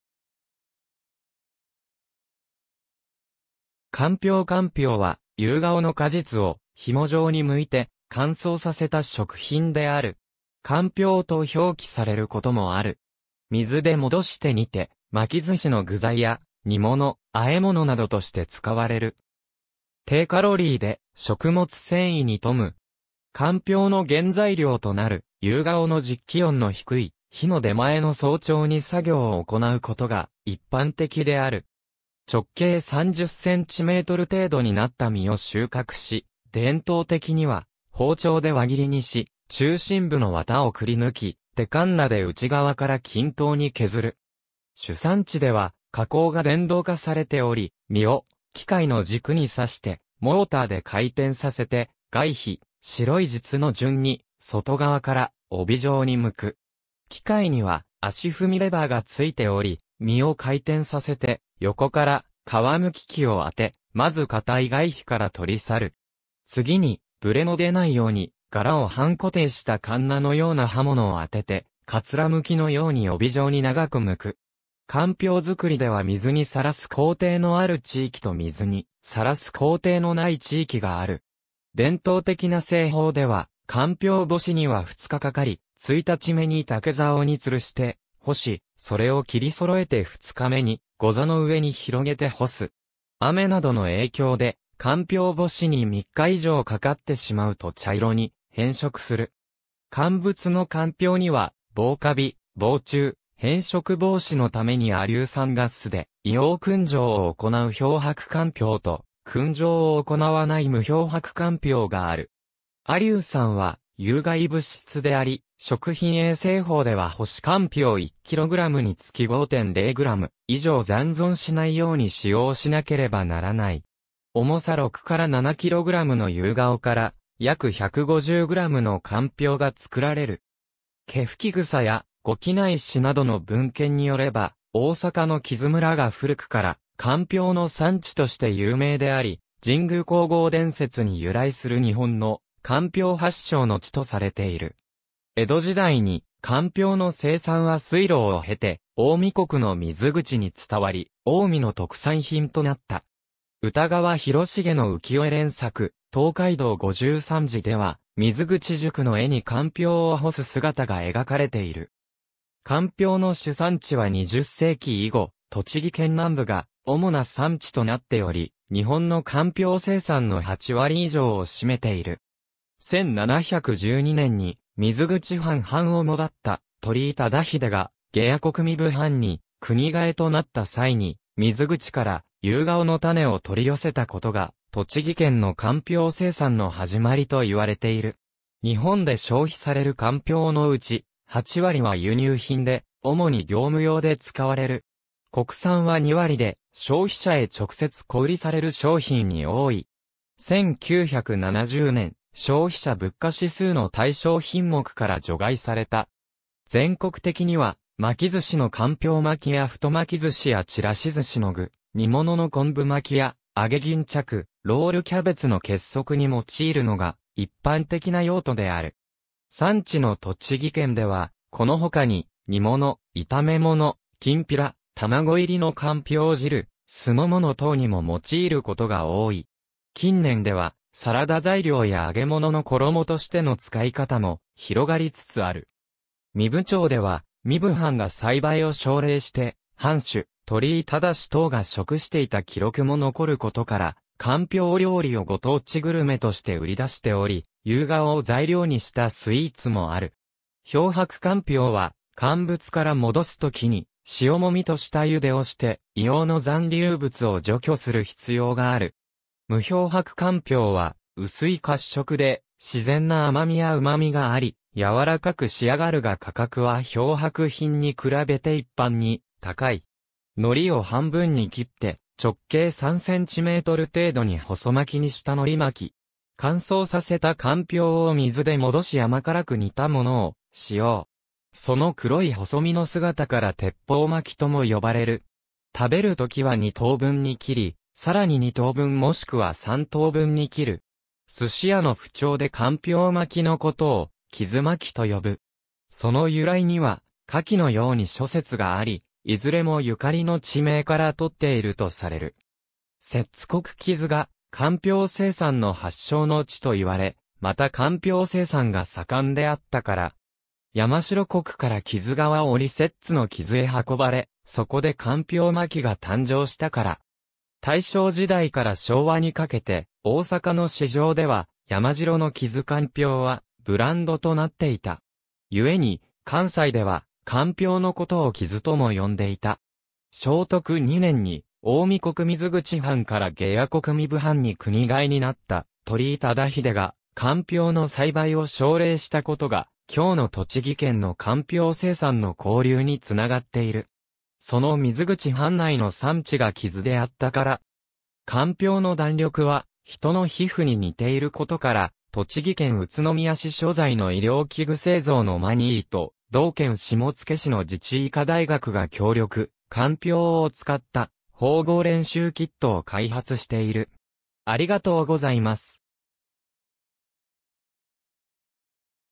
かんぴょう
干瓢 干瓢（かんぴょう）は、ユウガオの果実（ふくべ）を紐状に剥いて、乾燥させた食品（乾物）である。
「乾瓢」と表記されることもある。
水で戻して煮て、巻き寿司の具材や、煮物、和え物などとして使われる。
低カロリーで食物繊維に富む。
干瓢の原材料となるユウガオの実 気温の低い、日の出前の早朝に作業を行うことが一般的である。
直径30cm程度になった実を収穫し、伝統的には包丁で輪切りにし、中心部のワタをくりぬき、手鉋（てかんな）で内側から均等に削る。
主産地では加工が電動化されており、実を機械の軸に刺してモーターで回転させて、外皮、白い実の順に外側から帯状にむく。
機械には足踏みレバーが付いており、実を回転させて、横から皮むき器を当て、まず硬い外皮から取り去る。
次に、ぶれの出ないように、柄を半固定した鉋（かんな）の様な刃物を当てて、桂剥きのように帯状に長く剥く。
かんぴょう作りでは水にさらす工程のある地域（栃木県など）と水にさらす工程のない地域（福島県など）がある。
伝統的な製法では、かんぴょう干しには2日かかり、1日目に竹竿に吊るして干し、それを切り揃えて2日目にゴザの上に広げて干す。
雨などの影響でかんぴょう干しに3日以上かかってしまうと茶色に変色する。
乾物の干瓢には、防カビ、防虫、変色防止のために亜硫酸ガスで硫黄燻蒸を行う漂白干瓢と、燻蒸を行わない無漂白干瓢がある。
亜硫酸は有害物質であり、食品衛生法では干し干瓢1kgにつき5.0g以上残存しないように使用しなければならない。
重さ6 - 7キログラムのユウガオから、約150グラムの干瓢が作られる。
『毛吹草』や『五畿内志』などの文献によれば、大坂の木津村が古くからかんぴょうの産地として有名であり、神功皇后伝説に由来する日本のかんぴょう発祥の地とされている。
江戸時代にかんぴょうの生産は水路を経て近江国の水口（現在の滋賀県甲賀市）に伝わり、近江の特産品となった。
歌川広重の浮世絵連作『東海道五十三次』では、水口宿の絵に干瓢を干す姿が描かれている。
かんぴょうの主産地は20世紀以後、栃木県南部が主な産地となっており、日本のかんぴょう生産の8割以上を占めている。
1712年に水口藩藩主だった鳥居忠英が下野国（栃木県）壬生藩に国替えとなった際に、水口からユウガオの種を取り寄せたことが栃木県のかんぴょう生産の始まりといわれている。
日本で消費される干瓢のうち、8割は輸入品（中国産など）で、主に業務用で使われる。
国産は2割で、消費者へ直接小売りされる商品に多い。
1970年、消費者物価指数の対象品目から除外された。
全国的には、巻き寿司の干瓢巻きや太巻き寿司やちらし寿司の具、煮物の昆布巻きや揚げ巾着、ロールキャベツの結束に用いるのが一般的な用途である。
産地の栃木県では、この他に、煮物、炒め物、金平、卵入りの干瓢汁（海苔入りは「かみなり汁」と呼ばれ学校給食で出される）、酢の物等にも用いることが多い。
近年では、サラダ材料や揚げ物の衣としての使い方も広がりつつある。
壬生町では、壬生藩が栽培を奨励して藩主鳥居忠燾が食していた記録（『御献立帳』）も残ることから、かんぴょう料理をご当地グルメとして売り出しており、ユウガオを材料にしたスイーツもある。
漂白干瓢は乾物から戻す時に、塩揉みと下ゆでをして、硫黄の残留物を除去する必要がある。
無漂白干瓢は薄い褐色で、自然な甘味や旨味があり、柔らかく仕上がるが価格は漂白品に比べて一般に高い。
海苔を半分に切って直径3センチメートル程度に細巻きにした海苔巻き。
乾燥させた干瓢を水で戻し甘辛く煮たものを使用。
その黒い細身の姿から鉄砲巻きとも呼ばれる。
食べるときは二等分に切り、さらに二等分もしくは三等分に切る。
寿司屋の符牒で干瓢巻きのことを「木津巻き」と呼ぶ。
その由来には下記のように諸説があり、いずれもゆかりの地名から取っているとされる。
摂津国木津が干瓢生産の発祥の地といわれ、また干瓢生産が盛んであったから。
山城国（京都府南部）から木津川を下り摂津の木津へ運ばれ、そこで干瓢巻が誕生したから。
大正時代から昭和にかけて大阪の市場では山城の木津干瓢はブランドとなっていた。
故に、関西では干瓢のことを木津とも呼んでいた。
正徳二年に近江国水口藩から下野国壬生藩に国替えになった鳥居忠英が、干瓢の栽培を奨励したことが、今日の栃木県の干瓢生産の興隆につながっている。
その水口藩内の産地が木津であったから。
かんぴょうの弾力は人の皮膚に似ていることから、栃木県...